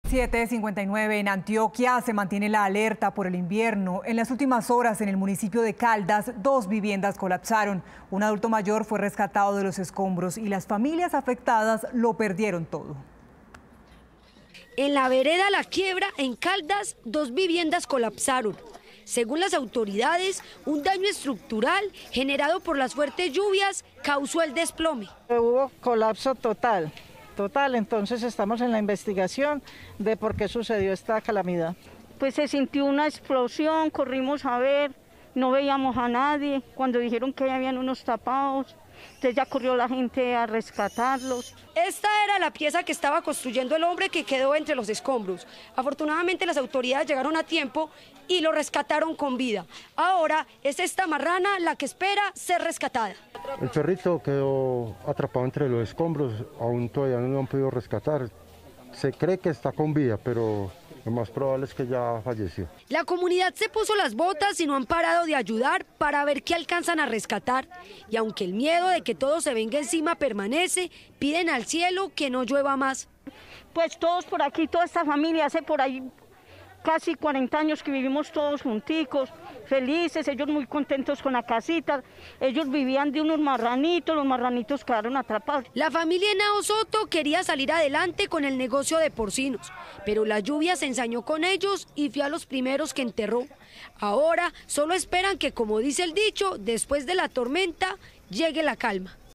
Rescatan a adulto mayor que quedó bajo escombros tras colapso de dos viviendas por lluvias
La emergencia se registró en Antioquia. Otra vivienda también se vino abajo debido a las fuertes precipitaciones y un hombre fue rescatado entre los escombros.